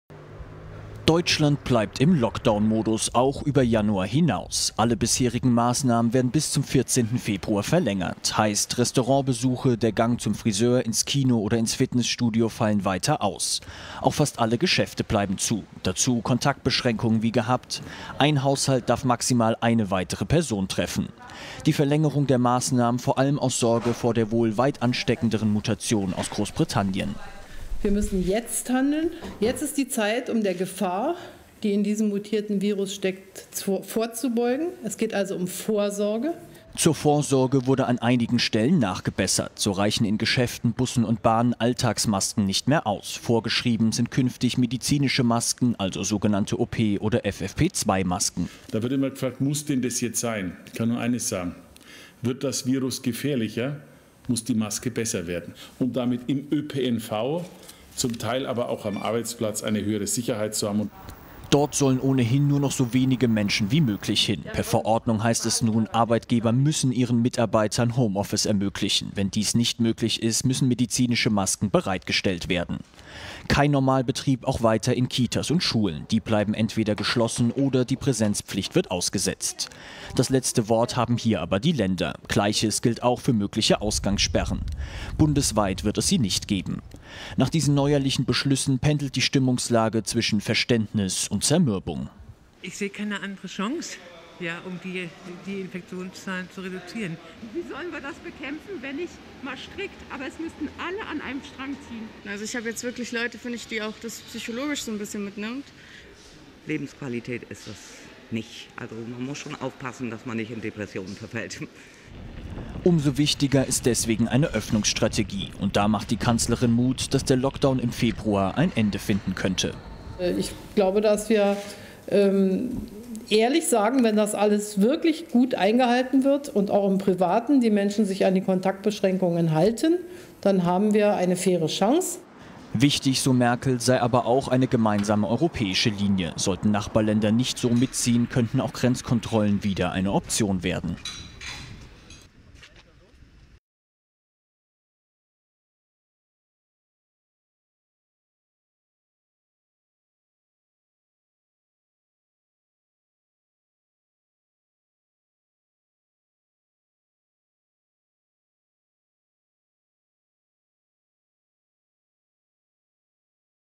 LOCKDOWN, MASKEN, SCHULEN, HOMEOFFICE - Das sind die neuen CORONA-Maßnahmen
Nach zähem Ringen und stundenlangen Gesprächen ist es nun beschlossene Sache. Der Lockdown wird bis zum 14. Februar verlängert. Einzelne Regeln werden zudem noch verschärft. Der von vielen befürchtete "Harte Lockdown" ist allerdings ausgeblieben. Wir fassen zusammen, worauf sich Bund und Länder nun konkret geeinigt haben.